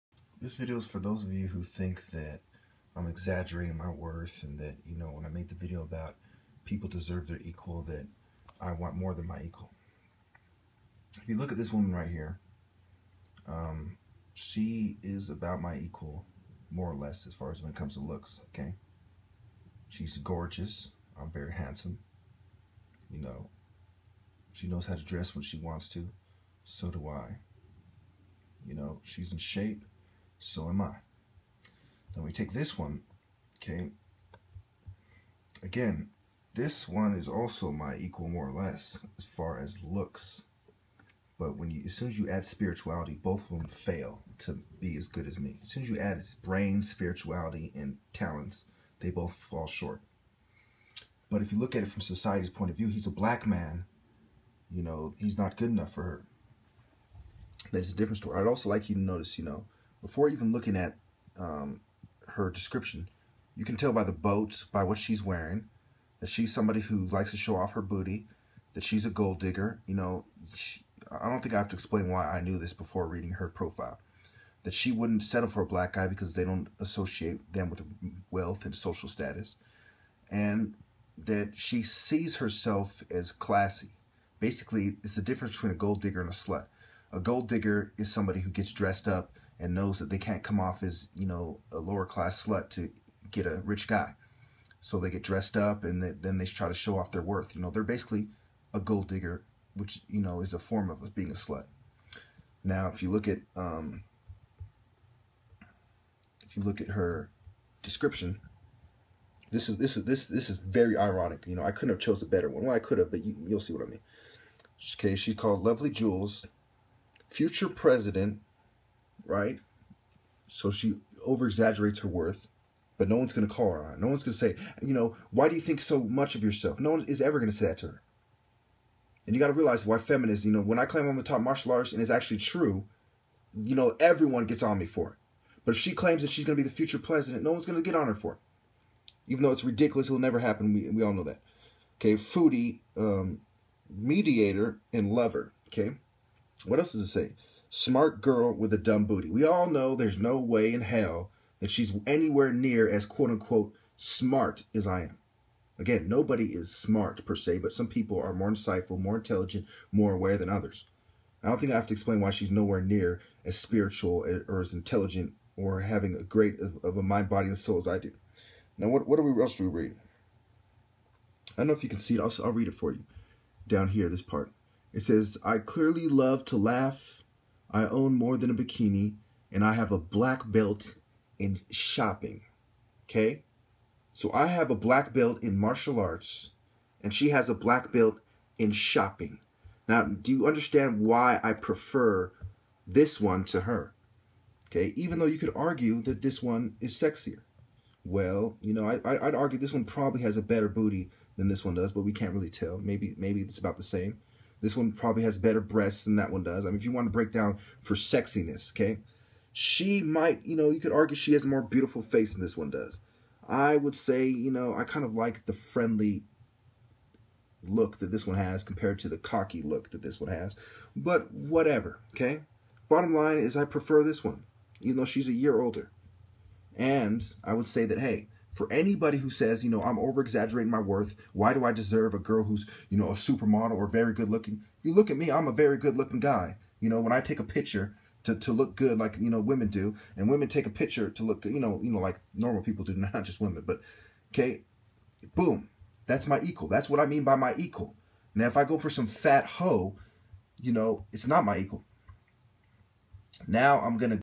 Exploring the type of woman I deserve
Before my oath all my exs should have been dimes and I should have been married to a dime without ever taking an oath. This is for all you ppl who think I am arrogant, or wrong.